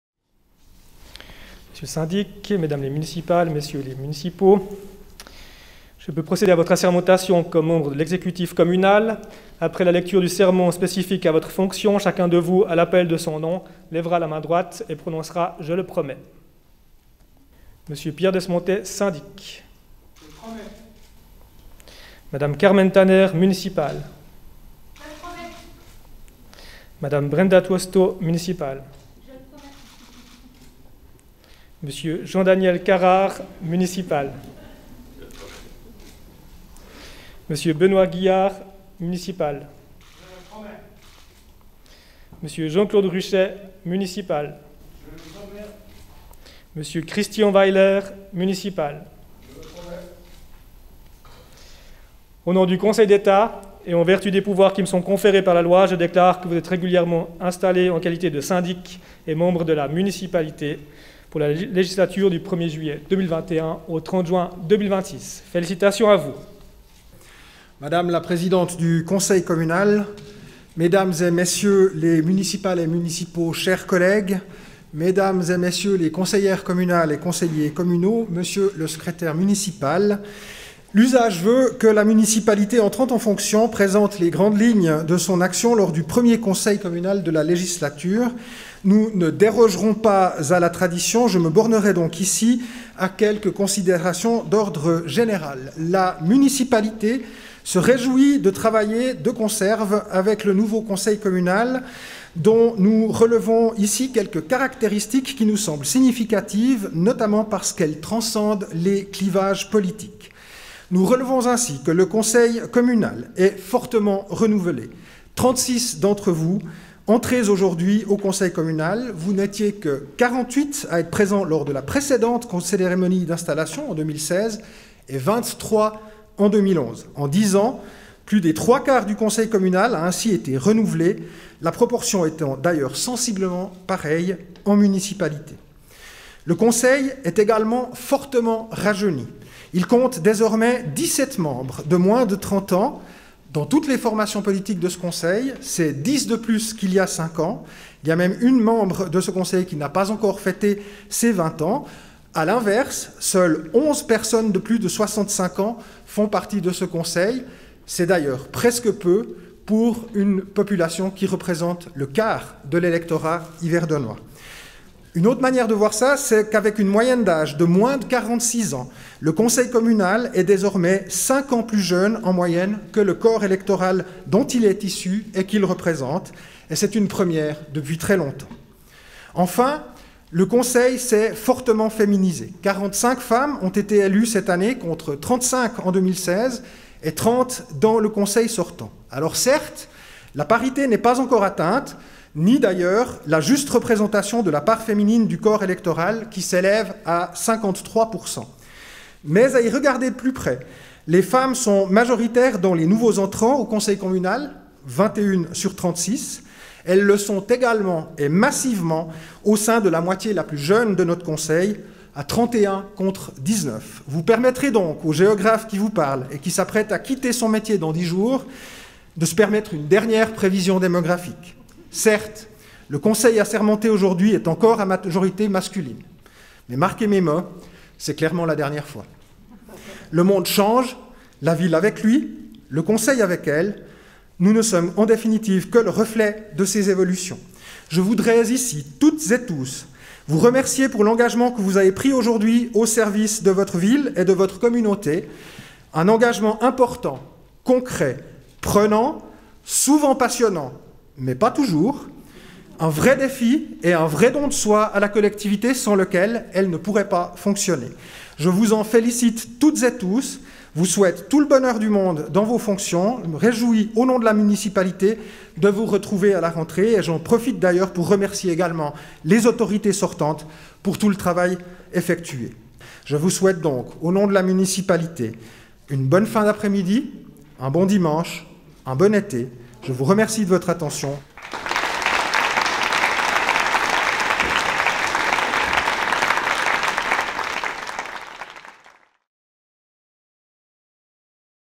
Ville d'Yverdon-les-Bains | Installation et assermentation des autorités communales.
Assermentation de la Municipalité et discours du syndic Pierre Dessemontet, à l’occasion de la cérémonie d’installation et d’assermentation des nouvelles autorités communales de la Ville d’Yverdon-les-Bains du 19 juin 2021 à la Marive. L'entier de la cérémonie peut être visionnée sur la chaîne DartfishTV des Archives audiovisuelles d’Yverdon-les-Bains